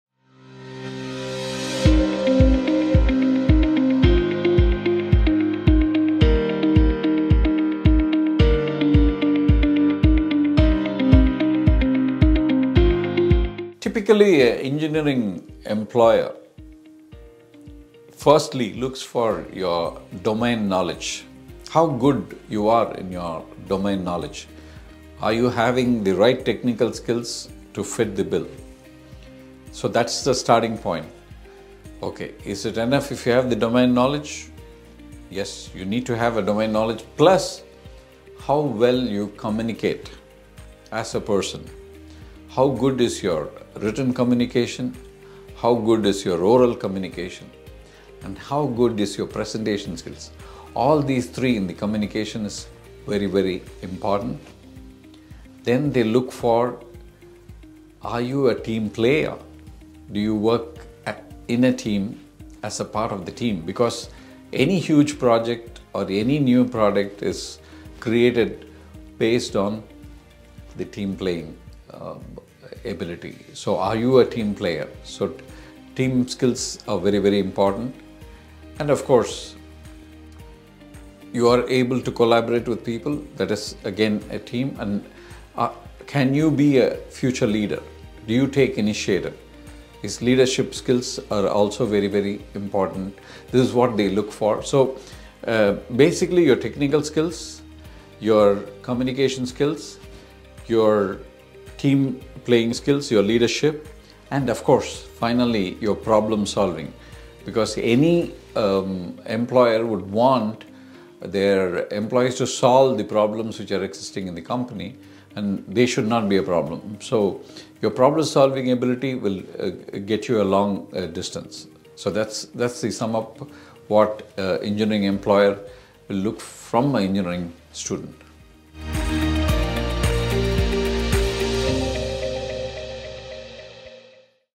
What skills do engineering employers look for in engineers?
With the competition for engineers growing every single day, engineering employers are in a fix to choose the best candidate for their company. So how do they manage this dilemma?

Did you know that employers look for specific skills when selecting a professional? Here in this video, we have one of our CADD Centre's leaders with enough industry experience brief you on the skills that companies look for.

Analyze if you have honed these skills, and if not, look for ways to enhance them so that you attain your dream job.